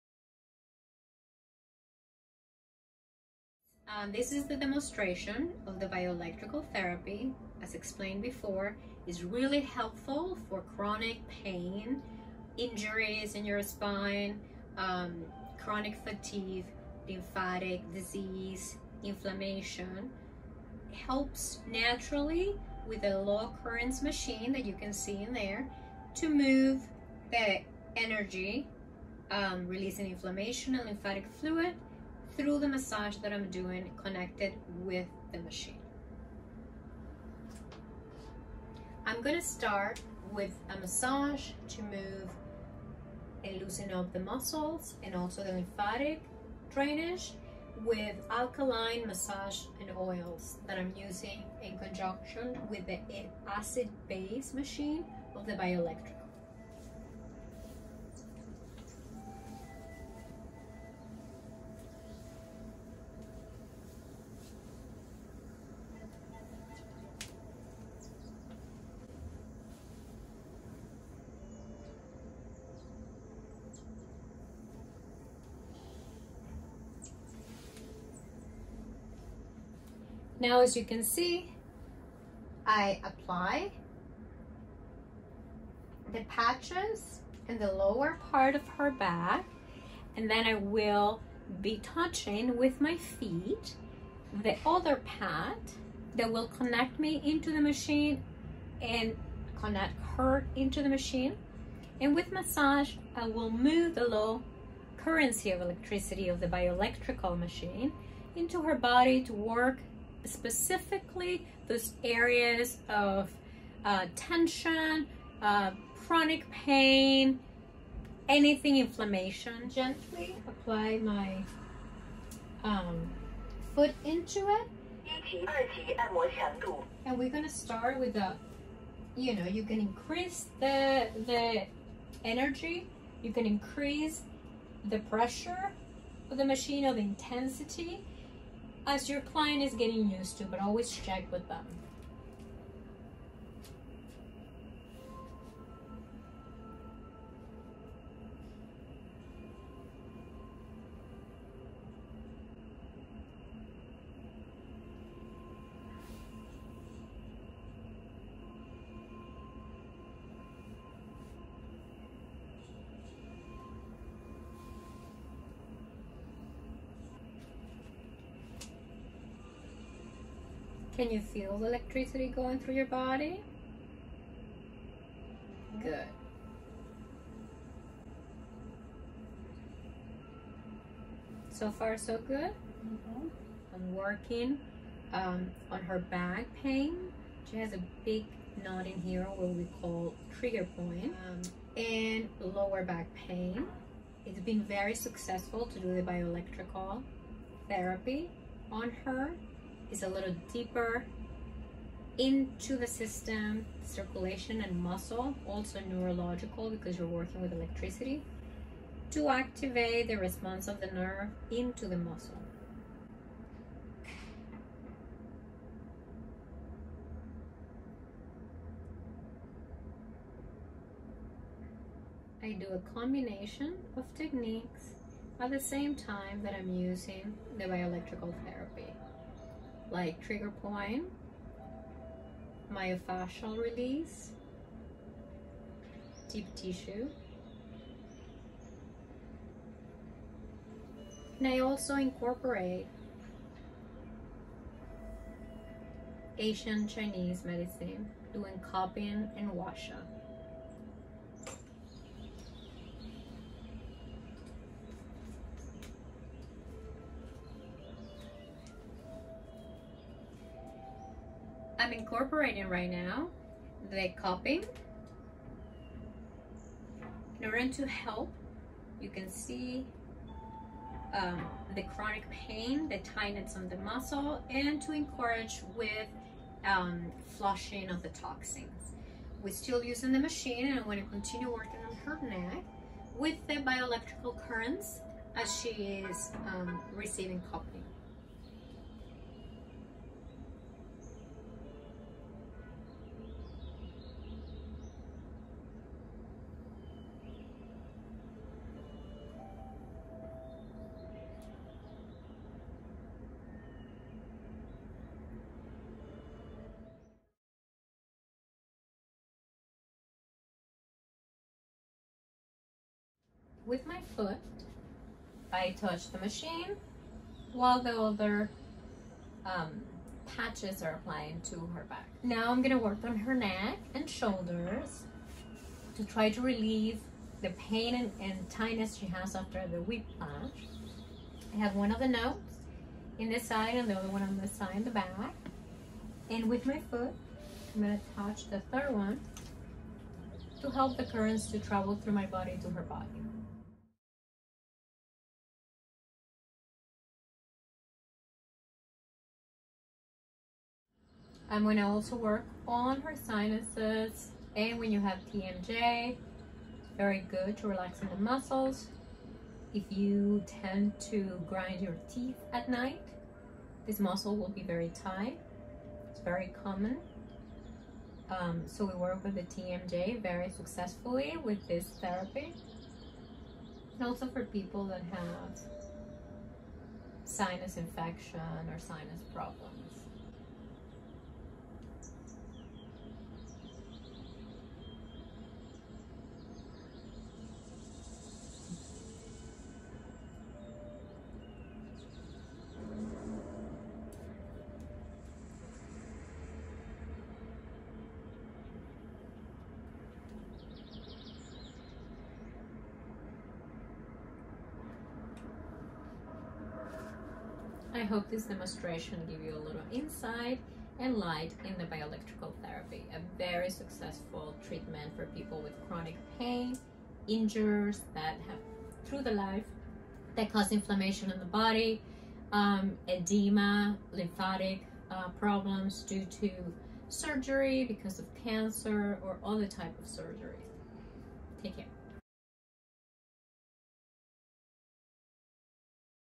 BIO ELECTRIC THERAPY now available at FabylousLiving!
At FabylousLiving, we are introducing a new type of therapy;
 BIO ELECTRIC THERAPY;
In this type of therapy, we utilize a machine generates a low electrical current which is transferred into the body through the therapist’s hands to help open and clear the body’s main meridian lines.
This type of therapy uses the body’s natural bioelectricity to stimulate nerves and cells and increases energy. Blood vessels expand creating a significant improvement in blood flow circulation and an increase in oxygen within the blood and throughout the body. This process helps to regulate and alkalize the body’s natural pH level. The therapy improves digestion, activates the auto-immune system, and helps to maintain good health.